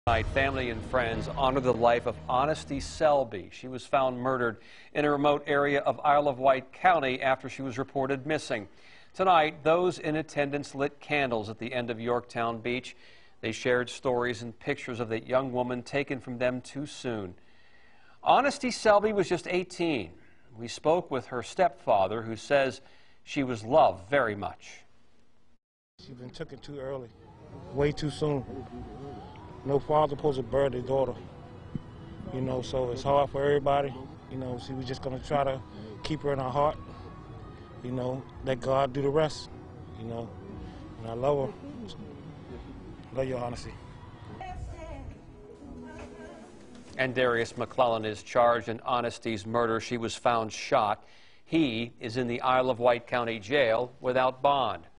Vigil held for Aonesty Selby
A vigil was held at Yorktown Beach for 18-year-old Aonesty Selby, who was found last Friday on a remote logging path in the 13000 block of Blue Ridge Trail just two days after her family in Williamsburg last heard from her.